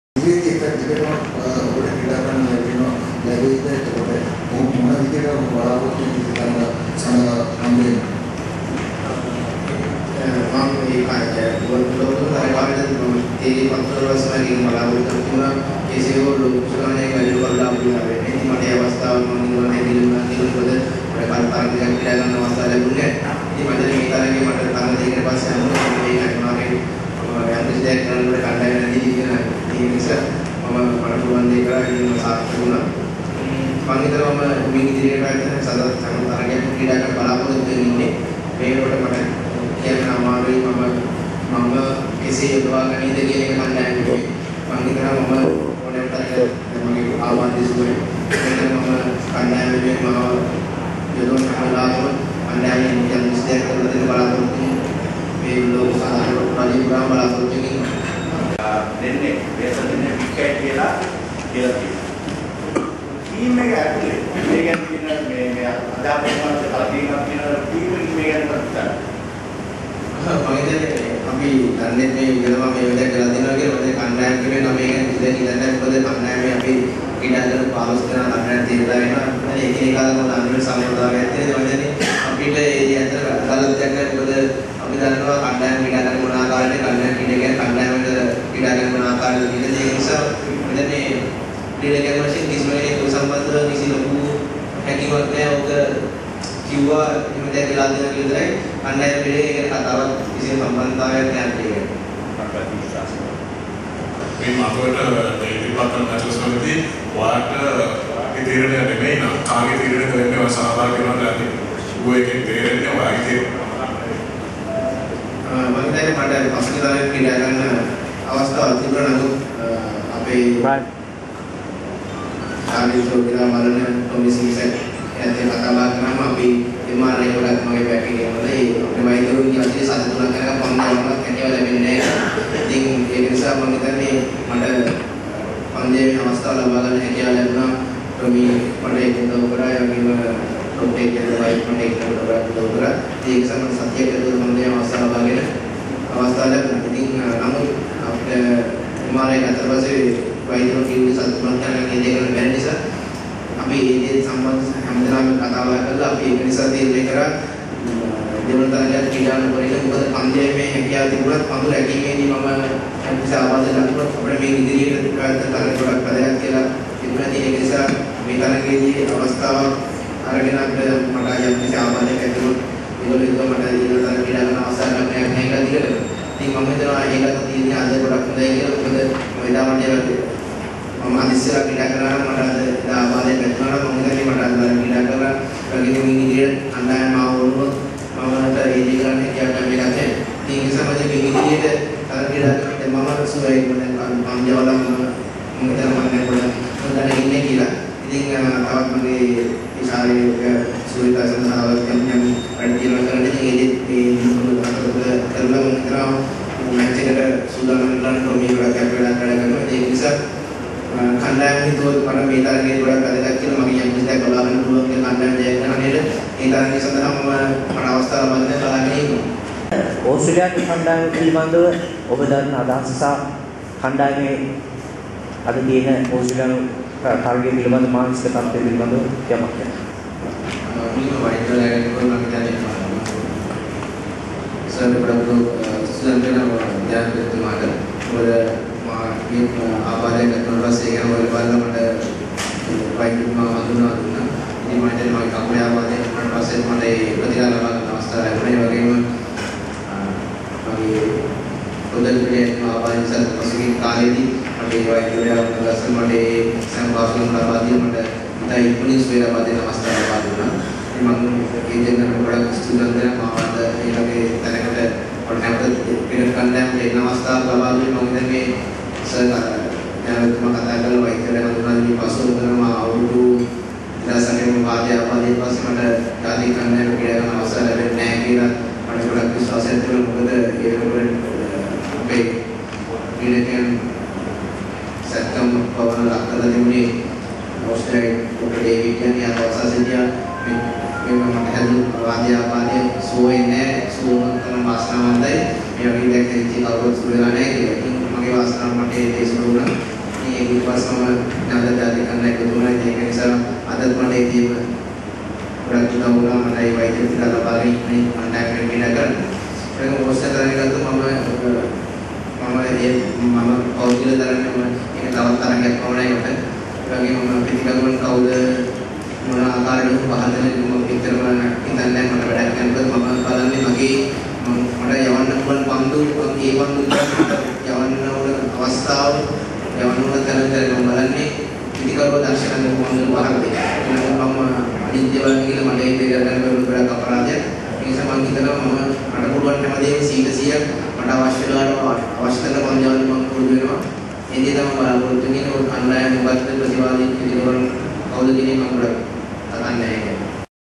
Sri Lanka vs Kenya ICC world cup 2011 After match press conference Lasith Malinga - Part 2
ICC Cricket World Cup 2011 - Match 14

 SRI LANKA v KENYA

Tuesday 1 March 2011 - Colombo

After match press conference Lasith Malinga

Malinga get's his 2nd hat-trick in a Cricket World Cup